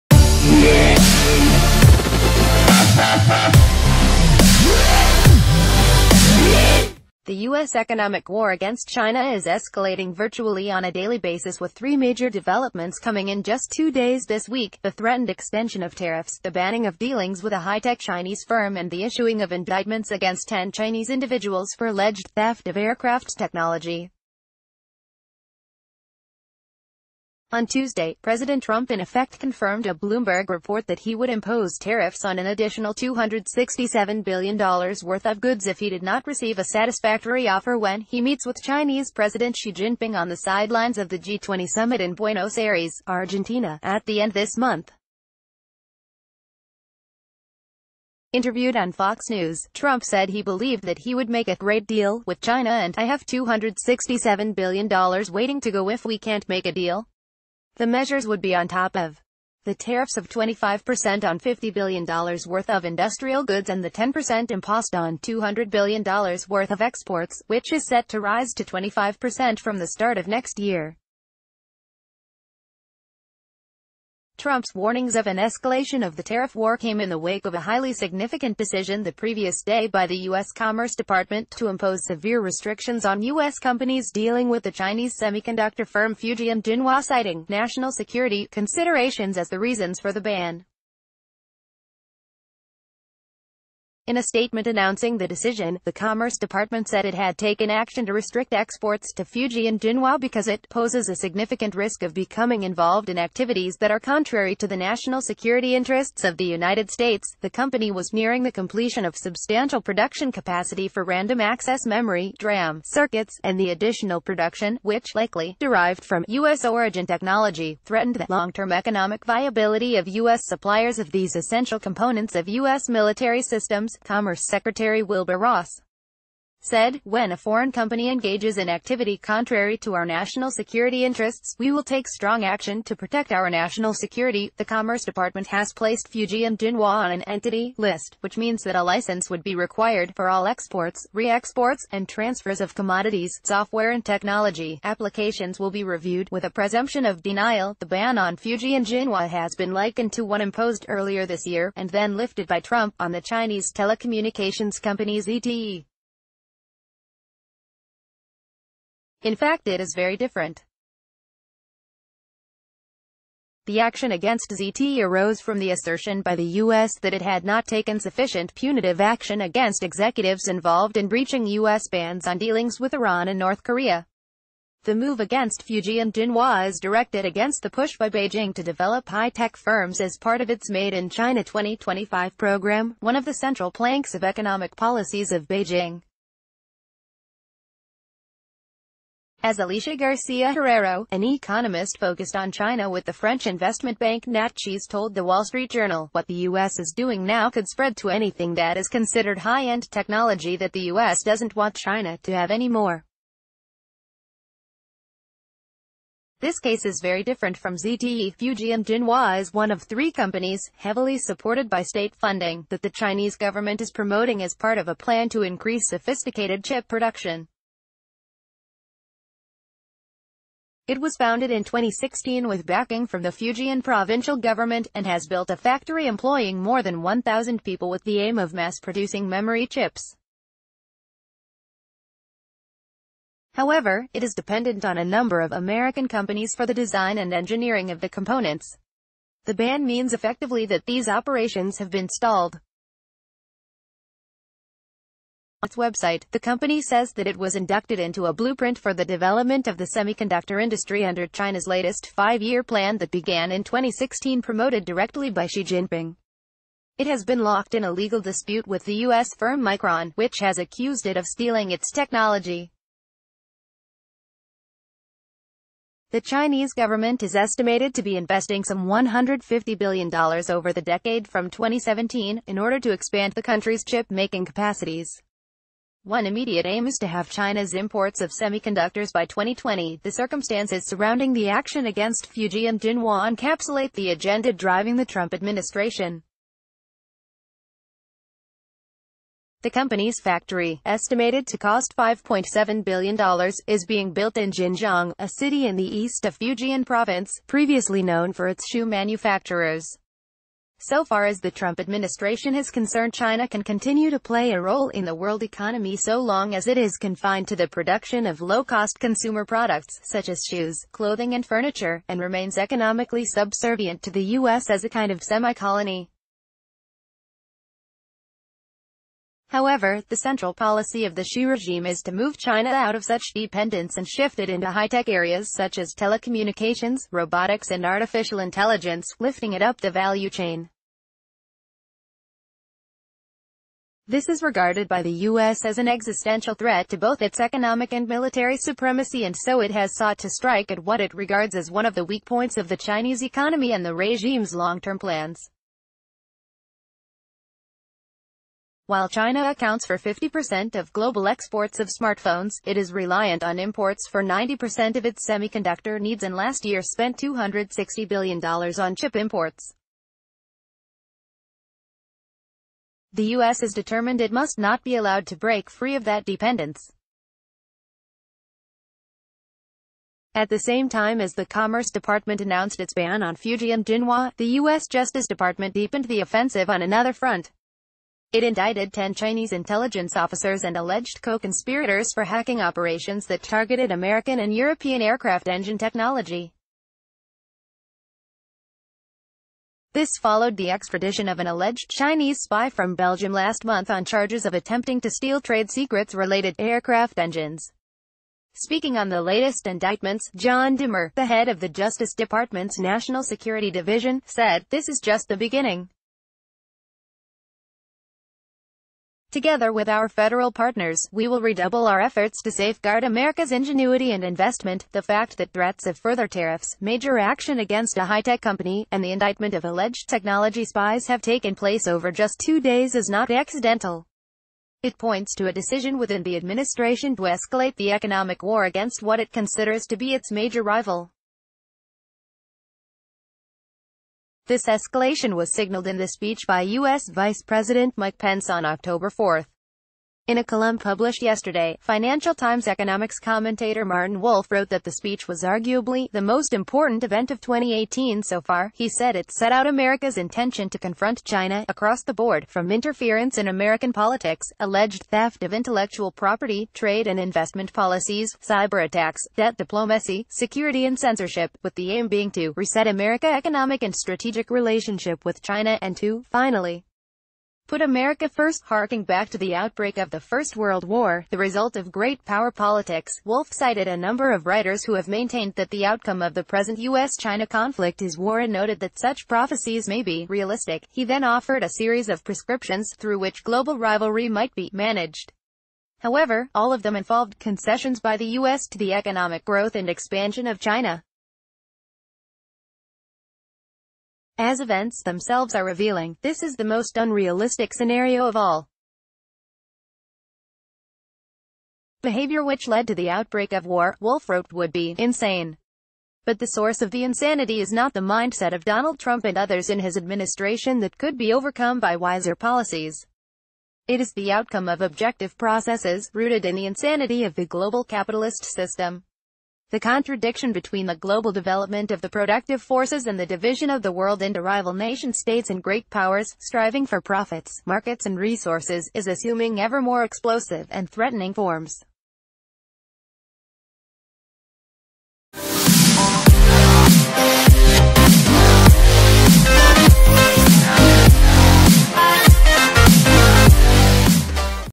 Daily News - A major escalation in the US economic war against China
The US economic war against China is escalating virtually on a daily basis with three major developments coming in just two days this week: the threatened extension of tariffs, the banning of dealings with a high-tech Chinese firm and the issuing of indictments against ten Chinese individuals for alleged theft of aircraft technology. On Tuesday, President Trump in effect confirmed a Bloomberg report that he would impose tariffs on an additional $267 billion worth of goods if he did not receive a...